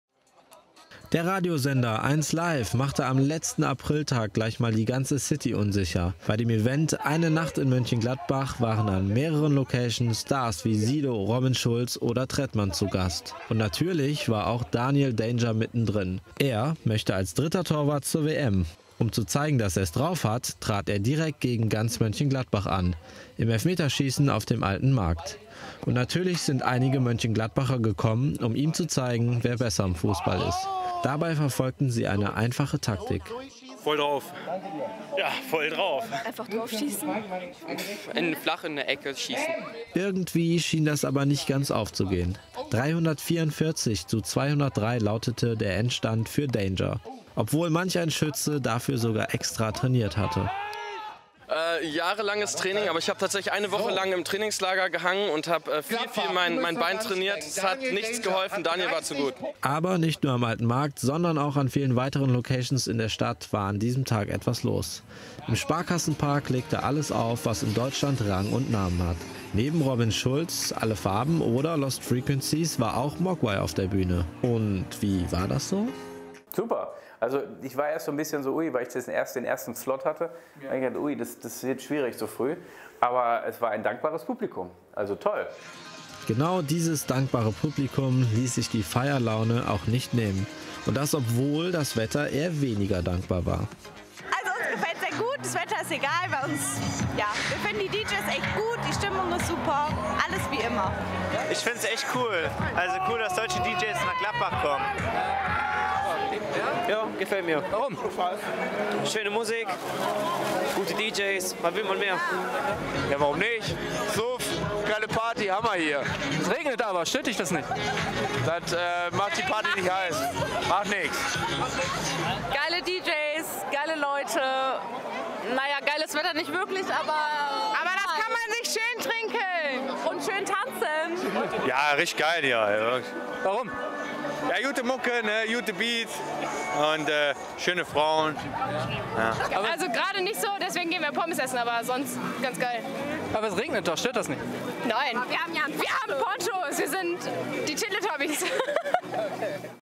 1LIVE Eine Nacht in Mönchengladbach mit Daniel Danger, Joris und MOGUAI
Wir waren vor Ort und haben uns mal umgesehen und den einen oder anderen Star getroffen...